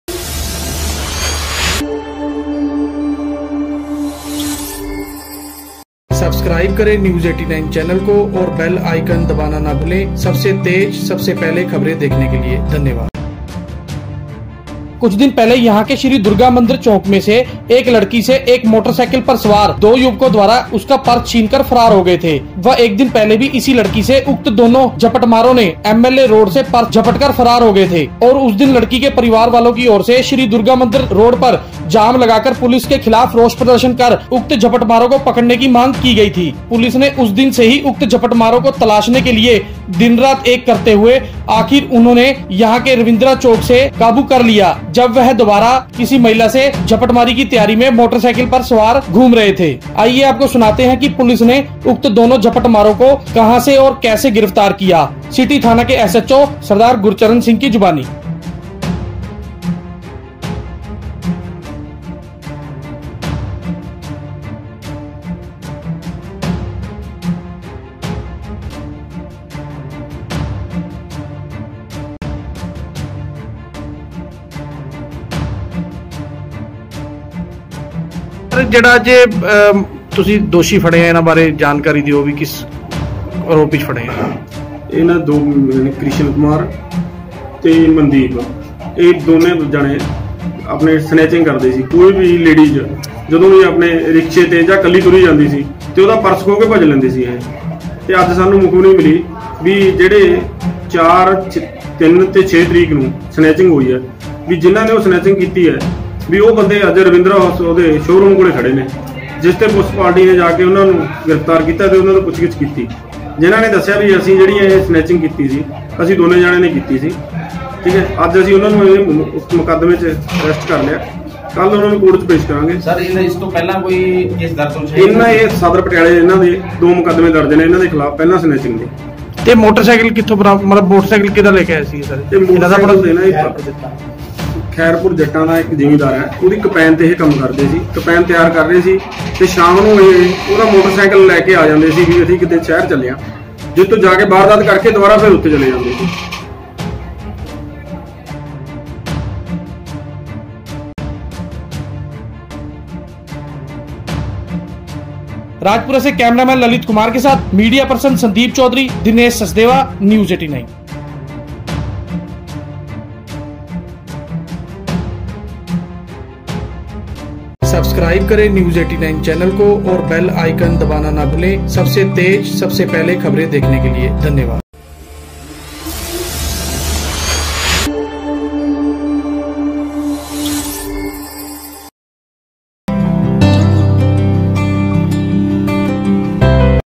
लो जी आखिर पुलिस ने पकड़ ही लिये महिलाओं से पर्स झपटने वाले दो झपटमार, देखो पूरी खबर
लो जी आखिर पुलिस ने पकड़ ही लिये महिलाओं से पर्स झपटने वाले दो झपटमार, देखो पूरी खबर  ਨਾਕੇਬੰਦੀ ਦੌਰਾਨ 2 ਝਪਟਮਾਰ ਕਾਬੂ